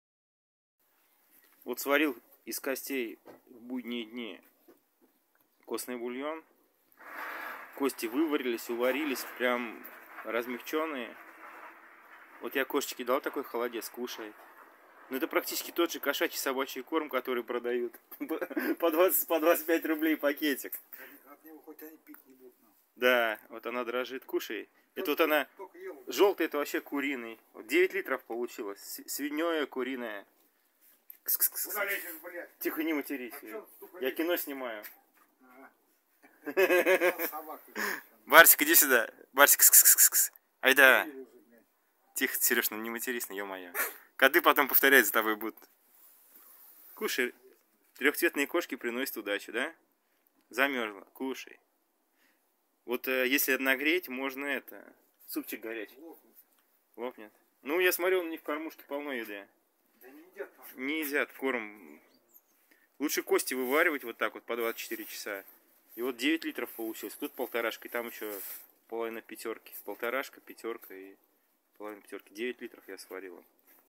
Кушать подано.
рецепт простой на восьмилитровую кастрюлю 2-3 кг костей. и воду, и варишь без всяких специй, если со специями, то можно самим употреблять очень вкусно.